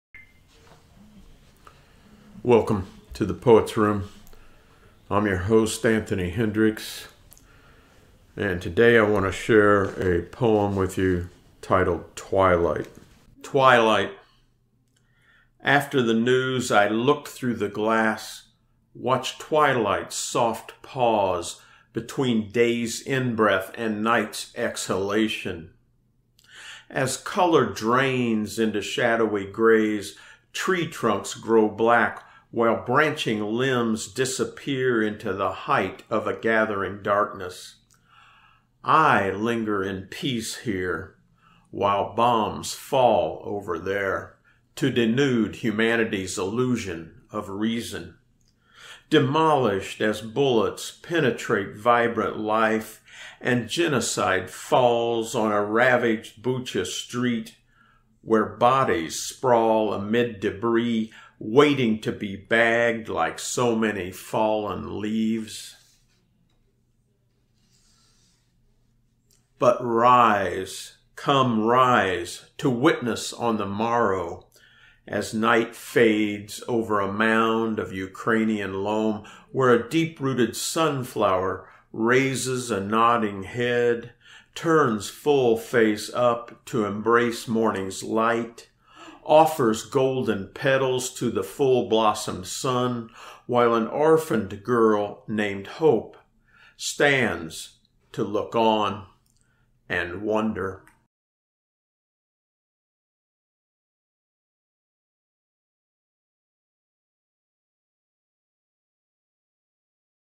Twilight in Ukraine
In this video I read the poem "Twilight" which starts serenely while observing twilight growing outside.  My muse led me then to connect the coming darkness with the tragedy growing in Ukraine.  I reflected then on the sun flower the National flower of Ukraine that symbolizes of desire to live free and grow in the light of democracy. In spite of all the suffering and death morning comes as a sun flower growing in a mound of Ukrainian loam opens to receive the light while an orphan girl named Hope looks on to wonder.